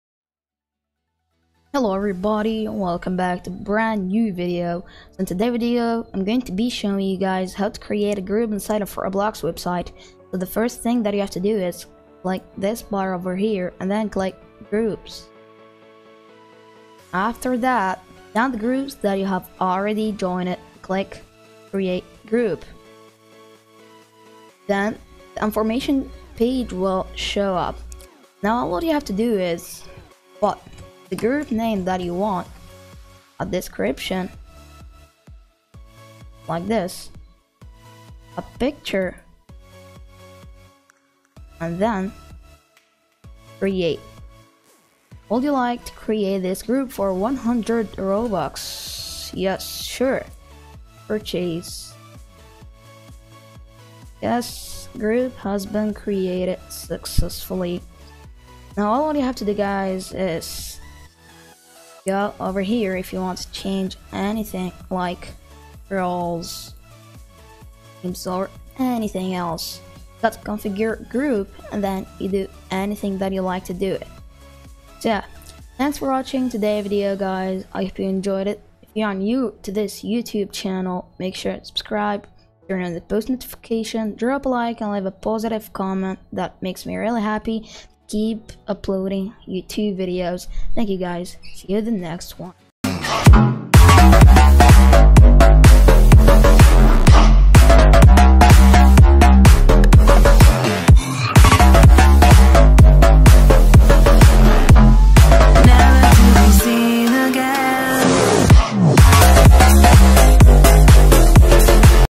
How To Make a Group On Roblox [Working]
Hey Guys , In Today Video Im Going To Be Showing You How To Make a

Group in Roblox Website , Don't Forget To Drop a Like !

🔔 Click On The Bell, And Turn On Your Notifications 🔔